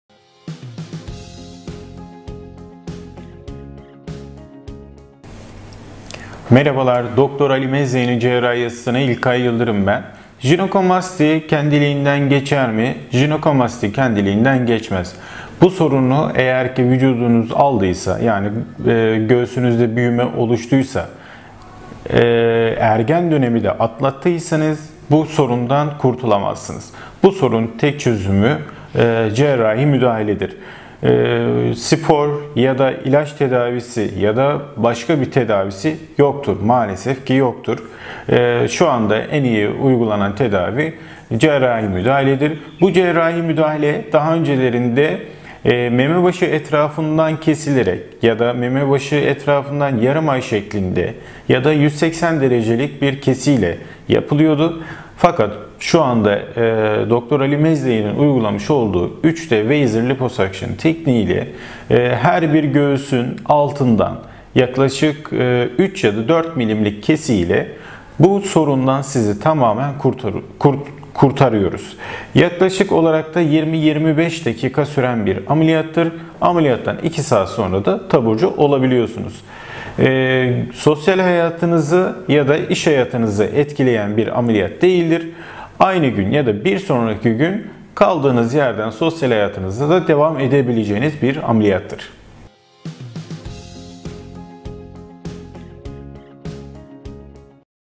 Jinekomasti Kendiliğinden Geçer Mi
İçerik : Jinekomasti ameliyatı genetik bir problemdir. Yani sonradan pek kazanılmaz. yağ dokusu glandüler doku dediğimiz dokunun büyümesidir.
Erkeklerde öztorejen hormonunda ki yükselme bu durumu tetikler. Bu yüzden ameliyat tek çözümdür. Spor veya diyet yapana da asla yapmayın demiyoruz.